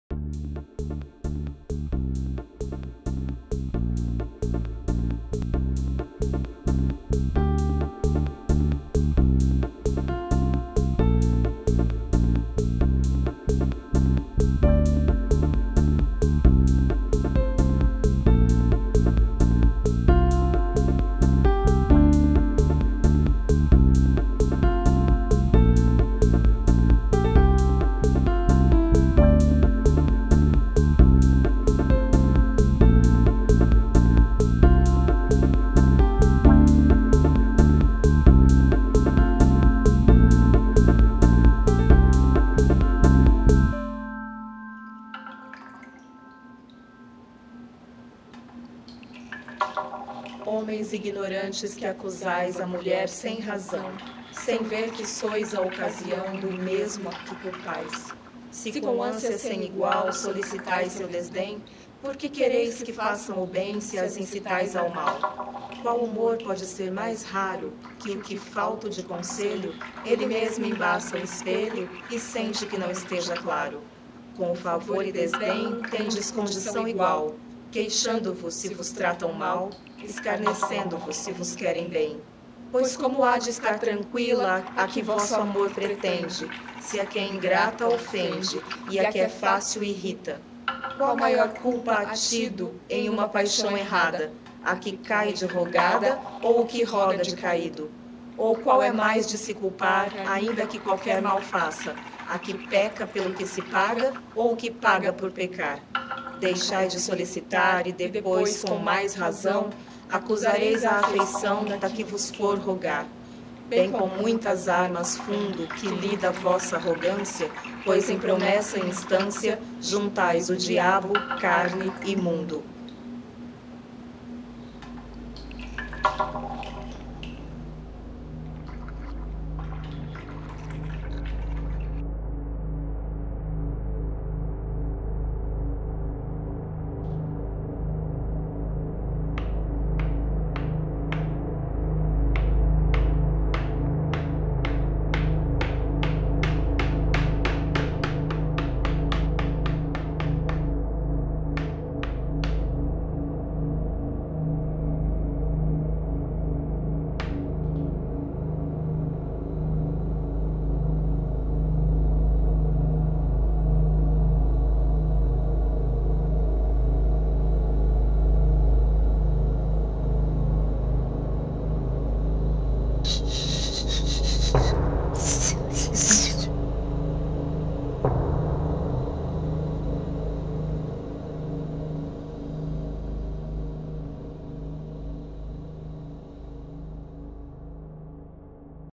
Trilha Sonora AO VIVO para a peça "A Rainha Da Danca" TEASER03
Peça Teatral:

“Sobre todas elas… Uma família tradicional brasileira qualquer e uma vida de anseios reprimidos. Há uma rainha dentro de todas nós esperando para ser coroada com a liberdade… Mas devemos ameaçar a ordem em nome da nossa pretensa felicidade? Gilda se libertará?“.